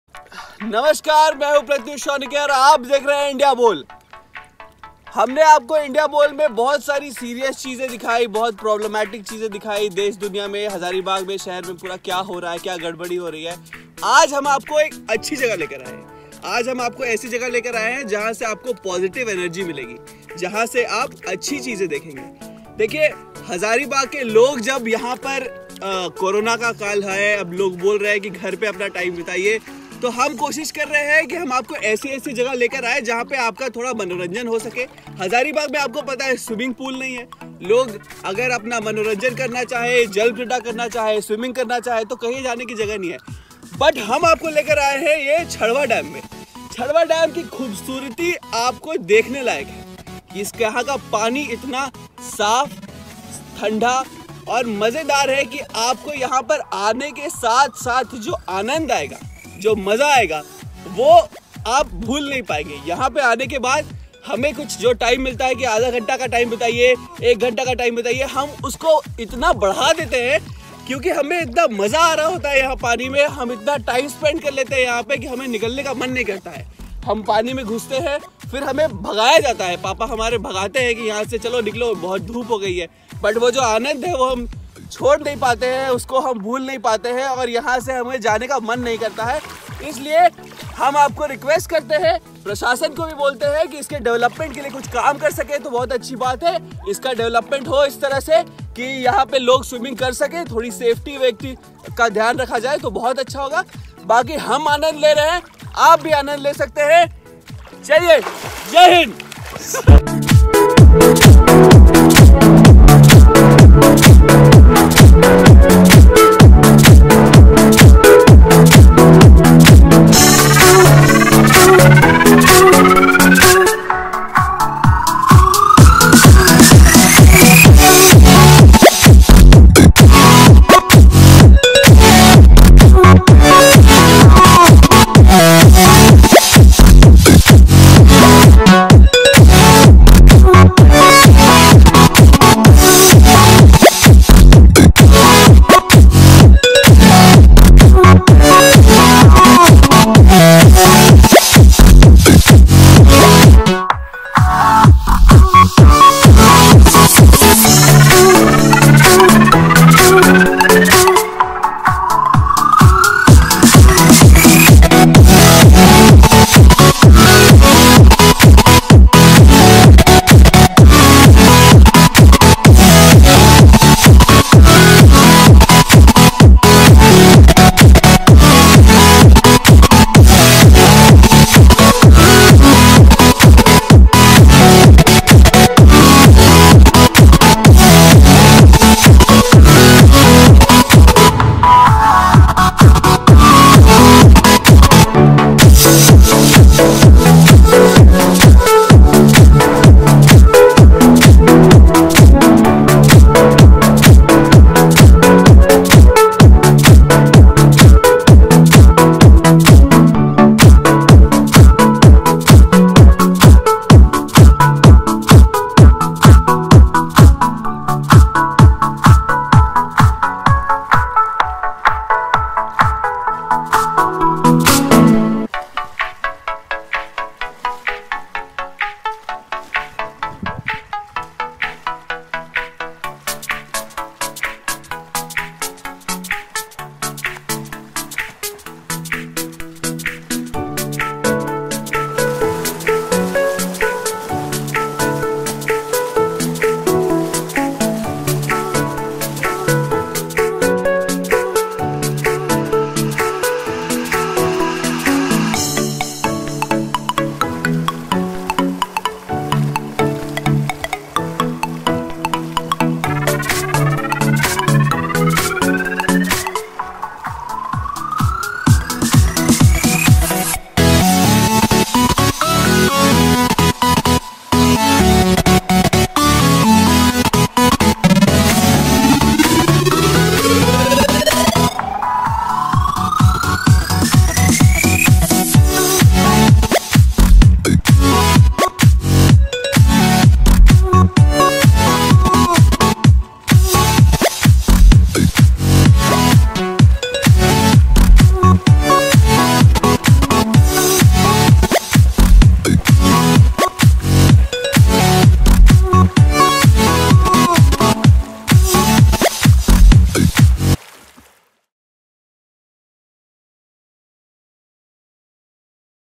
Swiming bol
हज़ारीबाग़ के छड़वा डैम में स्विमिंग का नजारा